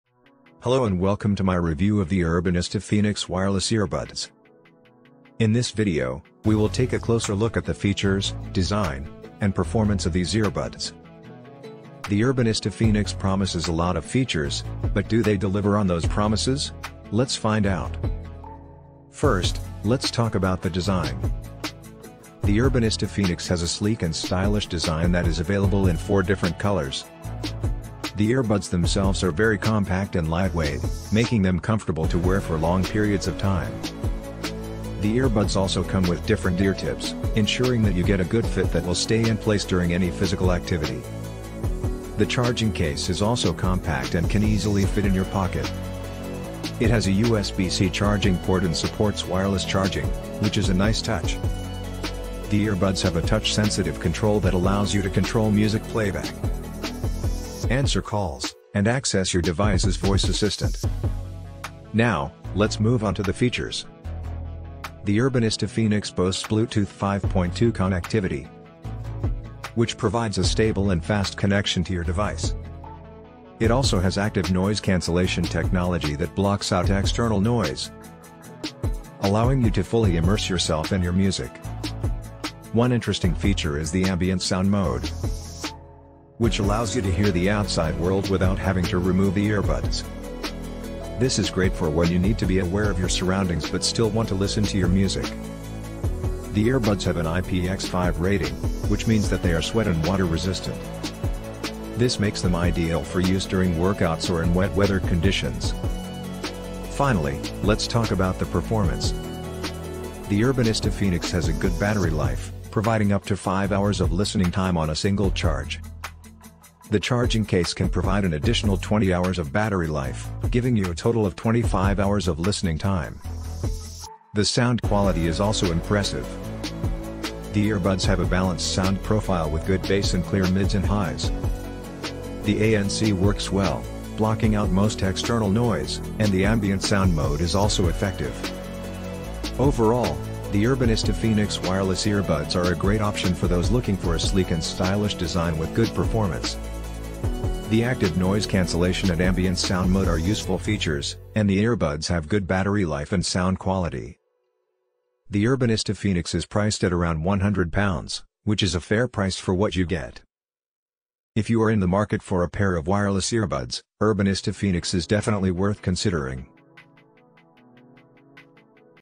Urbanista Phoenix: True Wireless Earbuds that Soar Review
Amazon Product Name : Urbanista Phoenix

Urbanista Phoenix Review: Looking for wireless earbuds with a long battery life? Check out this review of the Urbanista Phoenix, which boasts up to 9 hours of playtime and a charging case that can provide an additional 32 hours of battery life.